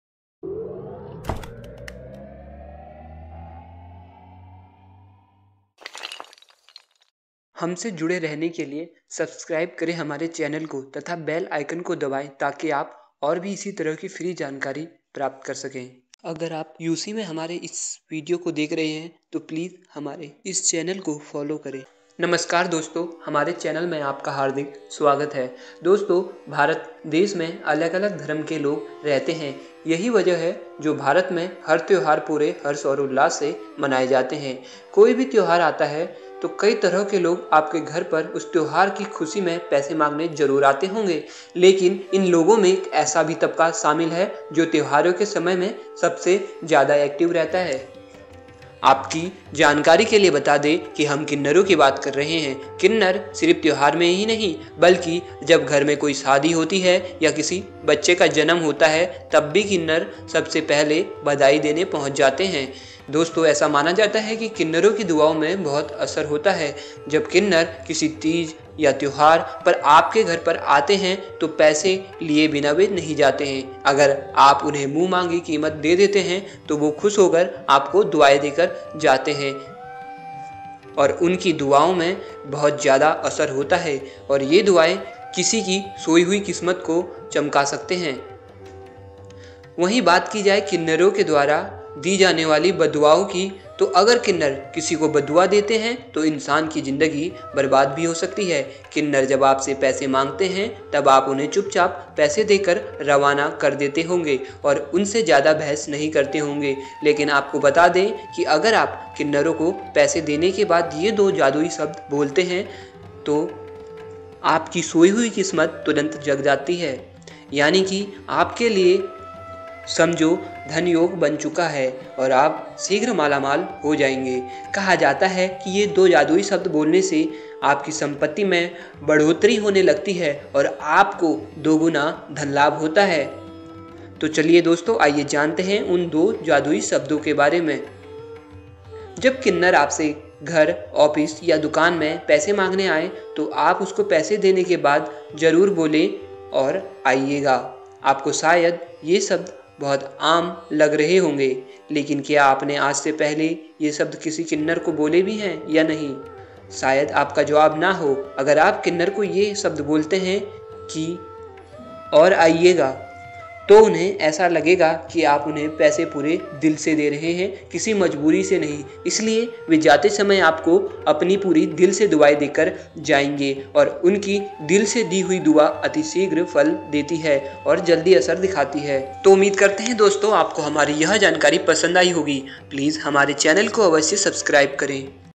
यदि कोई किन्नर आपको दिखे तो जरूर करना चाहिए विशेष काम होती है जीवन में बरकत
भारत में अलग अलग धर्म के लोग रहते है , यही कारण है की भारत में हर त्यौहार हर्षोउल्लास के साथ मनाया जाता है. कोई भी त्यौहार आता है तो आपके घर में कई तरह के लोग भी आते होंगे, कुछ पैसे लेने के लिए भी आते होंगे .